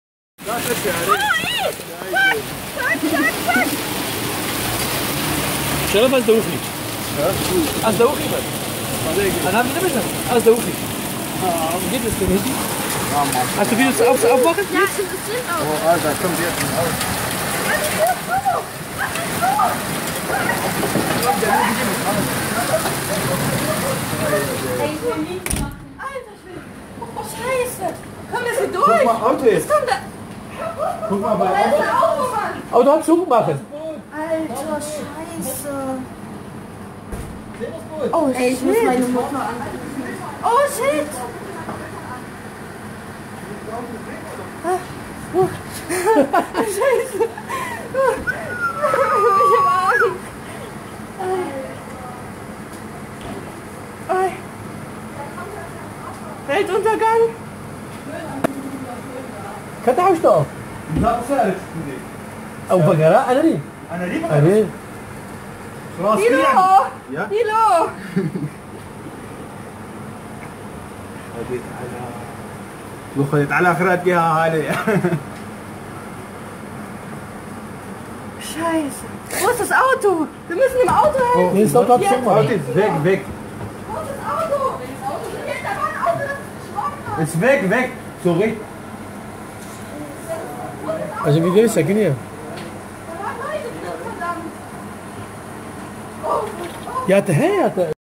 Schweres Gewitter mit über 50 L/m² - Bad Bibra | 19.5.2017
✼ Folgeninfo ✼
Schwere Gewitter waren das Fazit des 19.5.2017.
Nach 2 heißen Frühlingstagen mit 27° bis 31°C sind zum Nachmittag bis Nacht schwere Gewitter mit teils starkem Regen und Hagel bis ca. 5cm im Durchmesser vom Himmel gefallen.